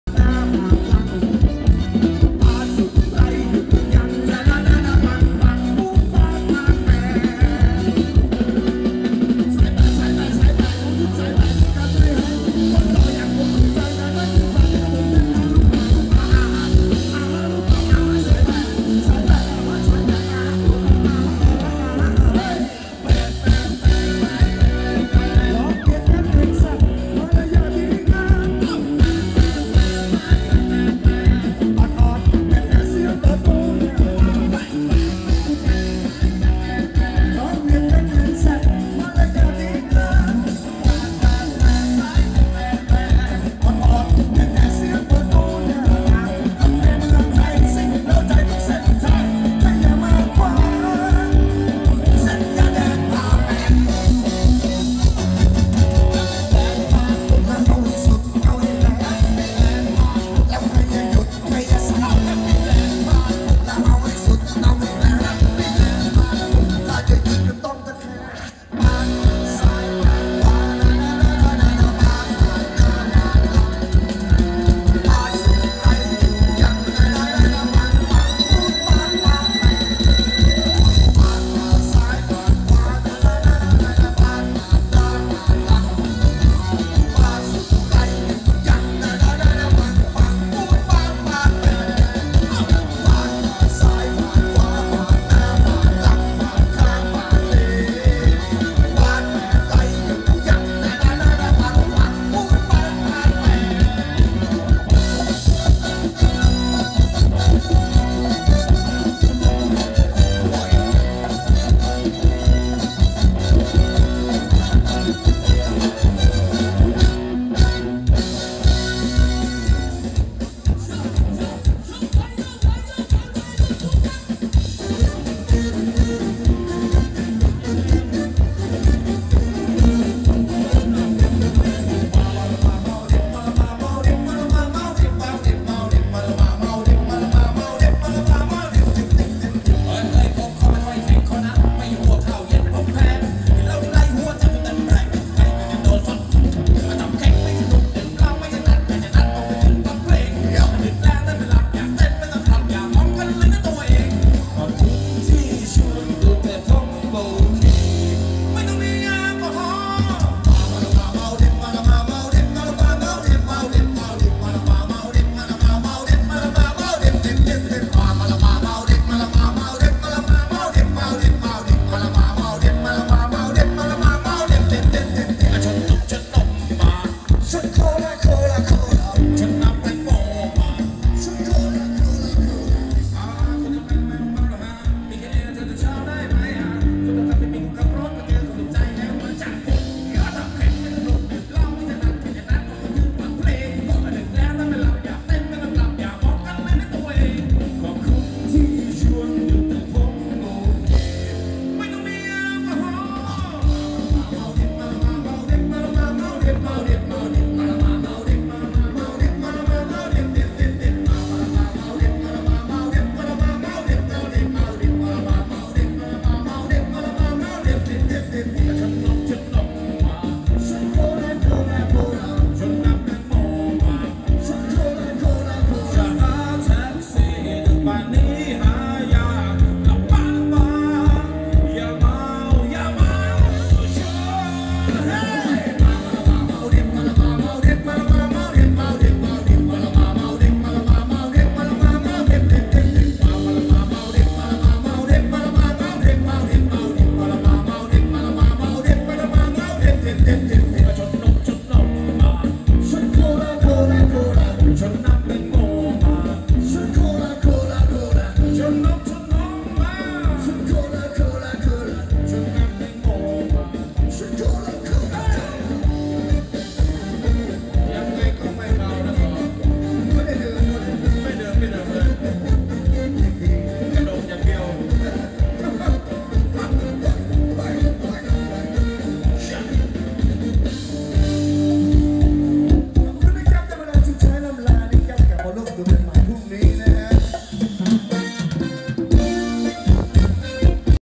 [CANON]【PhetchaburiDance Workout for weight loss】รำวงเพชรบุรี✿Health❤Kawaii❀ป๊าด8♡
name of Dancer 4 team : Leksriphet,Pernpit Wongjinda,Sanonoibanna,Daojarassang
name of Music Band : Jassmin
Show location : WatChorMuang,Phetchaburi,Near Hua Hin,Thailand